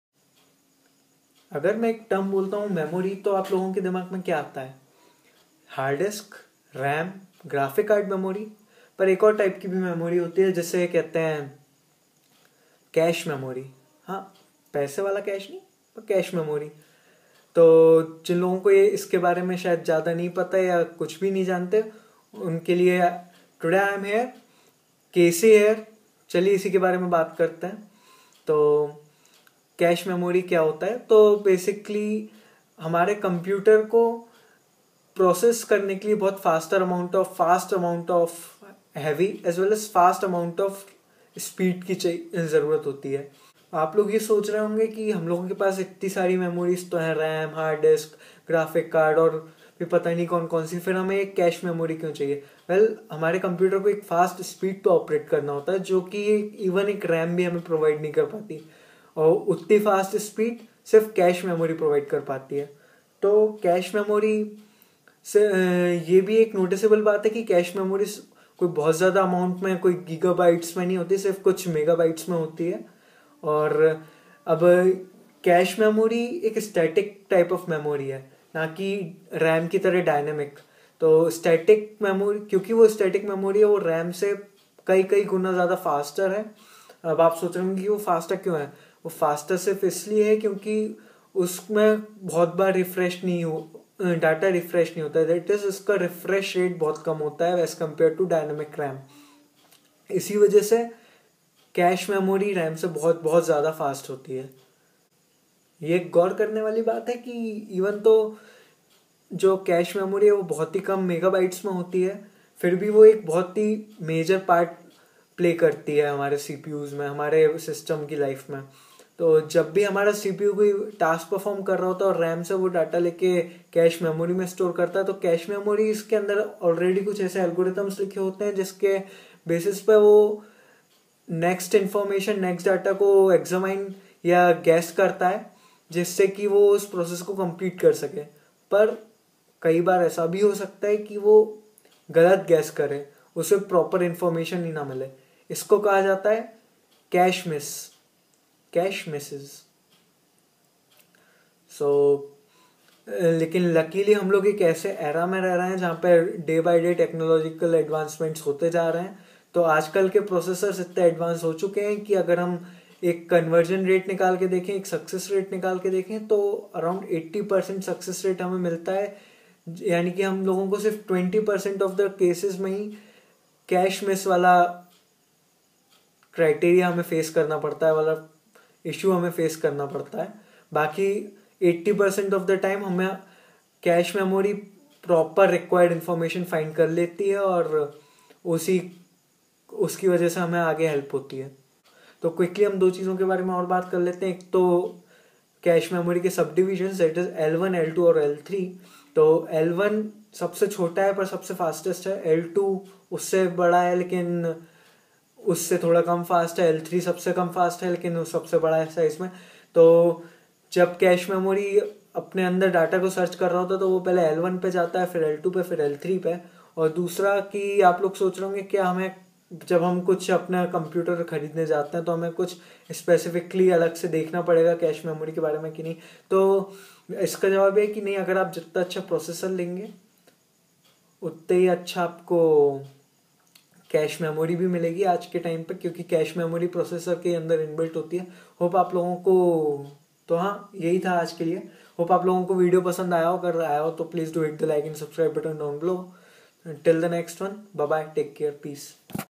WHAT IS CACHE MEMORY?? EXPLAINED !!!
one of the main memory in our computer system is cache memory but many of us don't really know what cache memory actually is what it does
well here is a quick  explaination